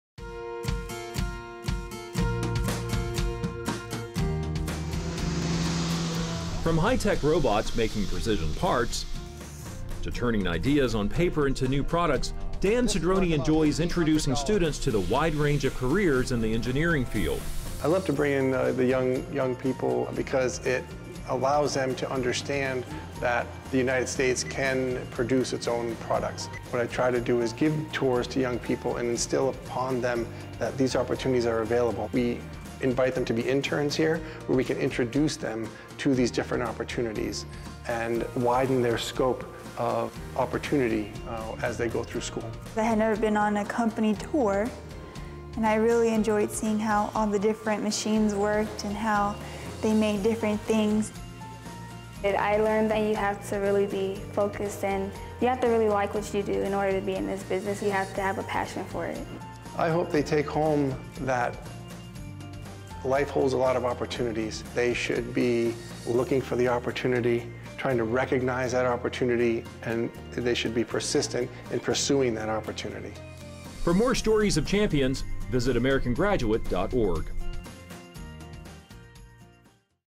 American Graduate Day 2017 - Dan Cedrone with Poly-Tech Industrial, Inc.
Dan Cedrone is the President of Poly-Tech Industrial, Inc., headquartered in Huntersville. From high-tech robots that make precision parts, to turning ideas on paper into new products, Cedrone enjoys introducing students to the wide range of careers in the engineering field.

“I love to bring in the young people and give them a tour around the manufacturing facilities because it allows them to understand that the United States can produce its own products,” and he added, “We can make things here in the U.S. as efficiently and competitively as other countries.”

Poly-Tech Industrial regularly offers student tours and Cedrone talks with the students about the opportunities available in the field.